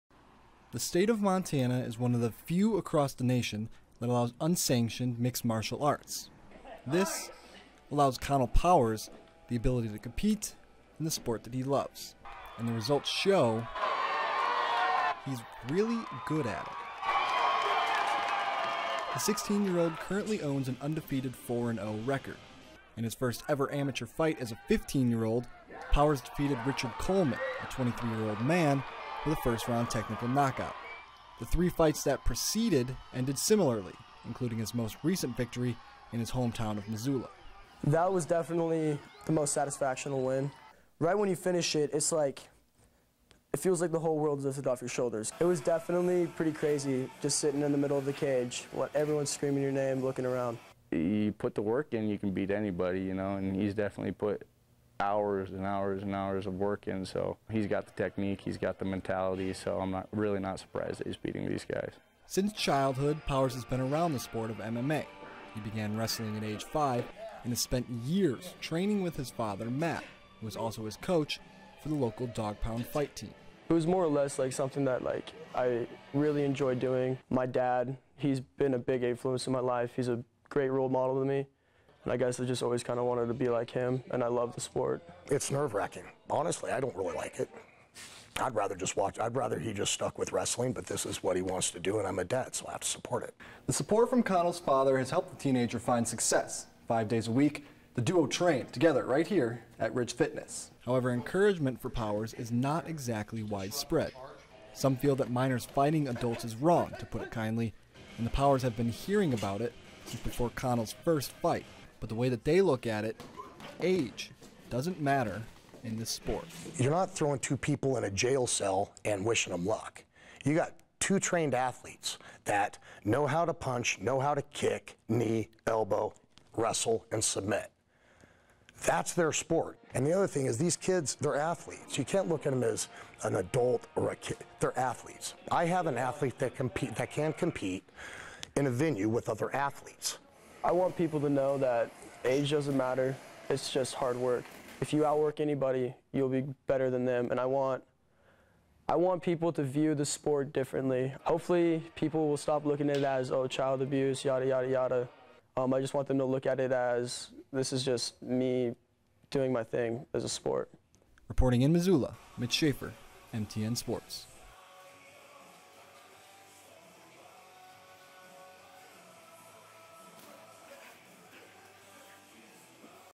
Age is only a number for Missoula mixed martial artist Conall Powers
Conall Powers may be "young" by mixed martial arts standards, but he has as much, if not more, experience than his elder opponents.